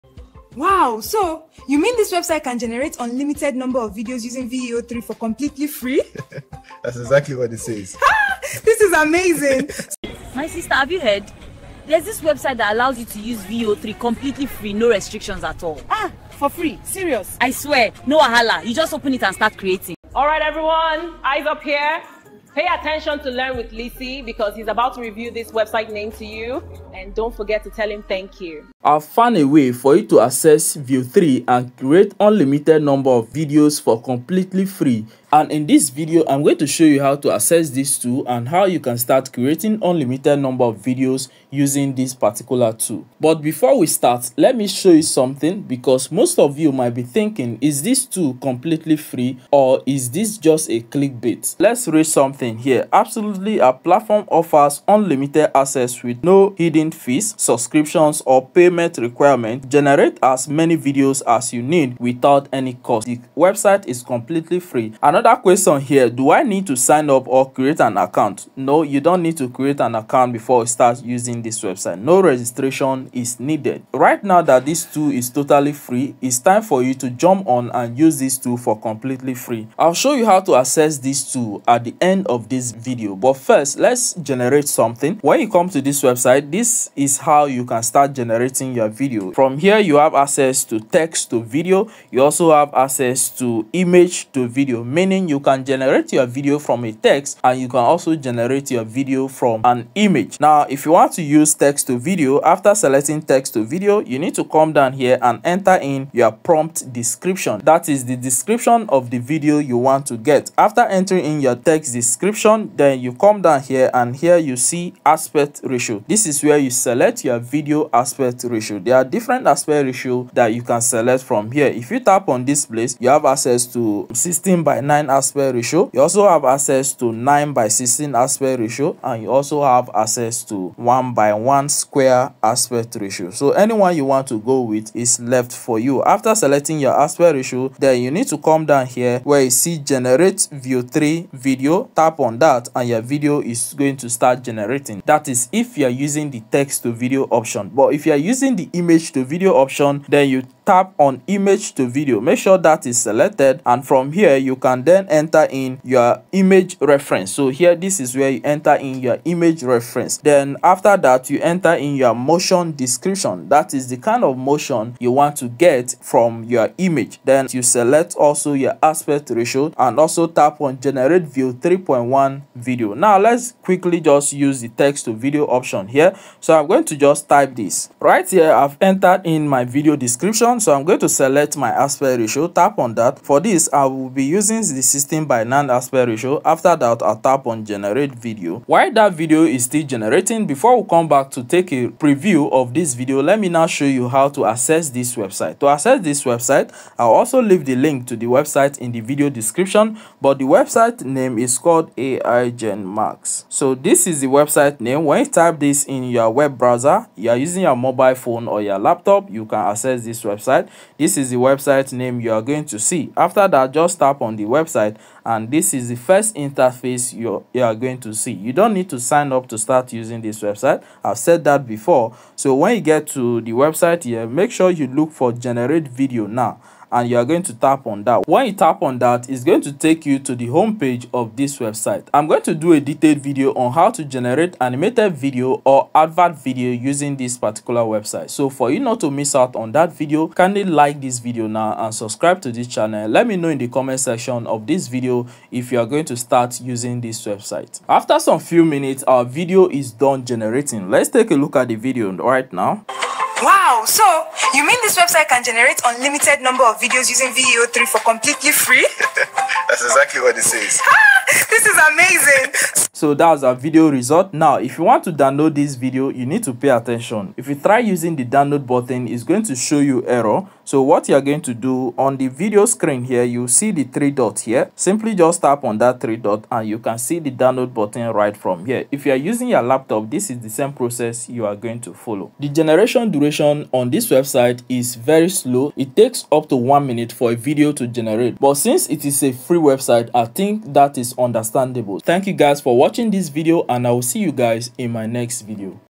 Google Just Made VEO 3.1 FREE for Everyone with Unlimited AI Video Generation (Don't miss out!)
Google VEO 3.1 for FREE with Unlimited AI Video Generation

In this video, I’ll show you how to access Google Veo 3.1 for free using AIGenMax. You’ll learn how to AIGenMax AI step by step to create AI Videos and make your AI films look smooth and professional. We’ll walk through the AIGenMax AI tutorial, explain how AIGenMax AI works, and show how to create full AI videos or faceless YouTube videos easily with AI. Whether you’re into AI filmmaking, storytelling, or content creation, this guide will help you create high-quality AI videos that look like real animation without any complicated tools.